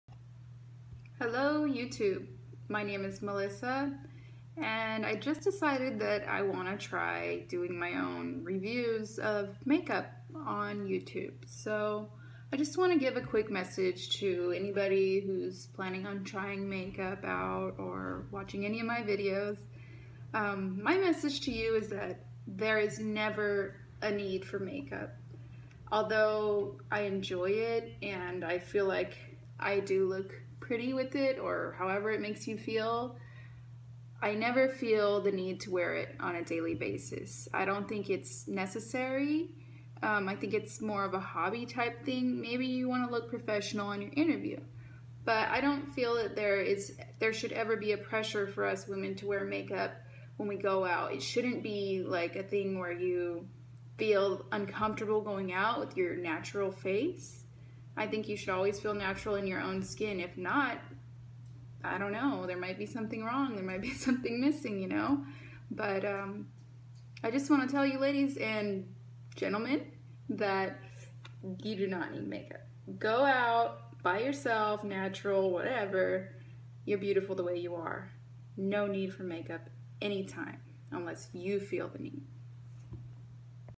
The Beginning
Hey Everybody. Please feel free to watch my first video introducing my Make up Try out Videos.  Yes I know I have pimples at this time and no you do not need to be a bully in any way type or form.  Bullies Beware!!  You will not be tolerated.

Thnks Every1!
Luv Missy- Melissa